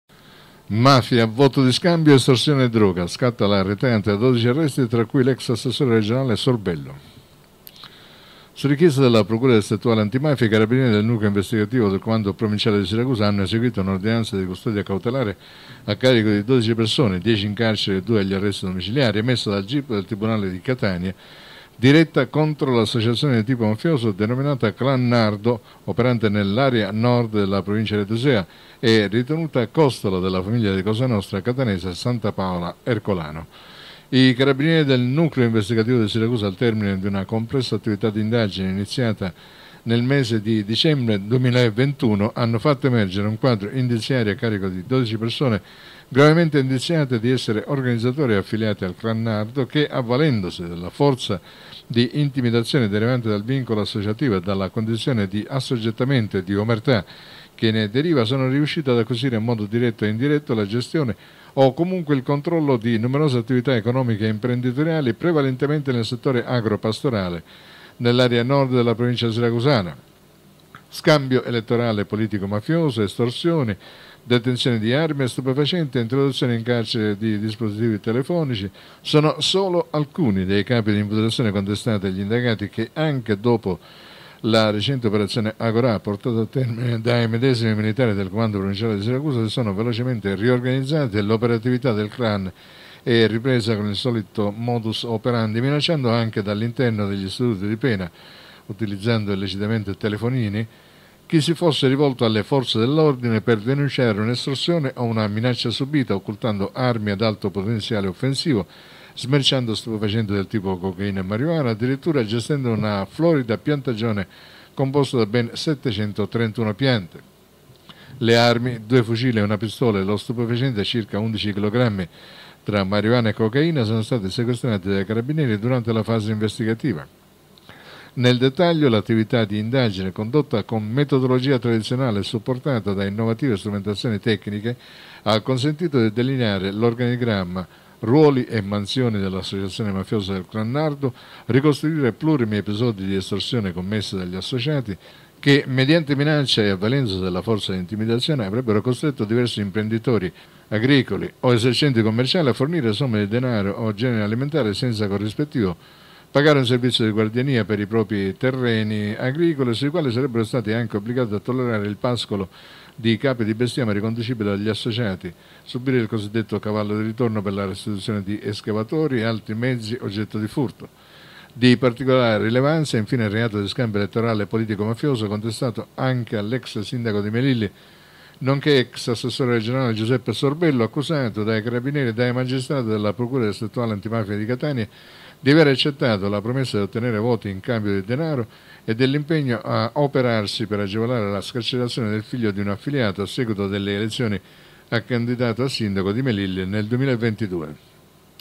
Mafia, voto di scambio, estorsioni e droga: blitz al clan Nardo, 12 arresti
Su richiesta della Procura Distrettuale Antimafia i Carabinieri del Nucleo Investigativo del Comando Provinciale di Siracusa hanno eseguito un’ordinanza di custodia cautelare a carico di 12 persone (10 in carcere e 2 agli arresti domiciliari) emessa dal Gip del Tribunale di Catania, diretta contro l’associazione di tipo mafioso denominata clan “Nardo” operante nell’area nord della provincia aretusea e ritenuta costola della famiglia di cosa nostra catanese “Santapaola Ercolano”.

I Carabinieri del Nucleo Investigativo di Siracusa, al termine di una complessa attività di indagine iniziata nel mese di dicembre 2021, hanno fatto emergere un quadro indiziario a carico di 12 persone gravemente indiziate di essere organizzatori ed affiliati al Clan “Nardo” che, avvalendosi della forza di intimidazione derivante dal vincolo associativo e dalla condizione di assoggettamento e di omertà che ne deriva, sono riusciti ad acquisire, in modo diretto e indiretto, la gestione o comunque il controllo di numerose attività economiche e imprenditoriali, prevalentemente nel settore agro-pastorale, nell’area nord della provincia siracusana.

Scambio elettorale politico/mafioso, estorsioni, detenzione di armi e stupefacenti, introduzione in carcere di dispositivi telefonici, sono solo alcuni dei capi di imputazione contestati agli indagati che, anche dopo la recente operazione “Agorà”, portata a termine dai medesimi militari del Comando Provinciale di Siracusa, si sono velocemente riorganizzati e l’operatività del clan è ripresa con il solito modus operandi, minacciando, anche dall’interno degli istituti di pena – utilizzando illecitamente telefonini – chi si fosse rivolto alle forze dell’ordine, per denunciare un’estorsione o una minaccia subita, occultando armi ad alto potenziale offensivo, smerciando stupefacenti del tipo cocaina e marijuana – addirittura gestendo una florida piantagione composta da ben 731 piante.

Le armi, due fucili e una pistola, e lo stupefacente, circa 11 kg tra marijuana e cocaina, sono stati sequestrati dai Carabinieri durante la fase investigativa.

Nel dettaglio, l’attività di indagine, condotta con metodologia tradizionale e supportata da innovative strumentazioni tecniche, ha consentito di delineare l’organigramma, ruoli e mansioni dell’associazione mafiosa del clan “Nardo”, ricostruire plurimi episodi di estorsione commessi dagli associati che, mediante minaccia e avvalendosi della forza di intimidazione, avrebbero costretto diversi imprenditori agricoli o esercenti commerciali a fornire somme di denaro o generi alimentari senza corrispettivo, pagare un servizio di “guardiania” per i propri terreni agricoli, sui quali sarebbero stati anche obbligati a tollerare il pascolo di capi di bestiame riconducibili agli associati, subire il c.d. “cavallo di ritorno” per la restituzione di escavatori ed altri mezzi oggetto di furto.

Di particolare rilevanza è infine il reato di scambio elettorale politico /mafioso contestato anche l’ex sindaco di Melilli, nonché ex assessore regionale, Giuseppe Sorbello, accusato dai carabinieri e dai magistrati della Procura distrettuale antimafia di Catania di aver “accettato la promessa di ottenere voti in cambio di denaro e dell’impegno ad operarsi per agevolare la scarcerazione del figlio di un affiliato” a seguito delle elezioni a candidato a sindaco di Melilli nel 2022.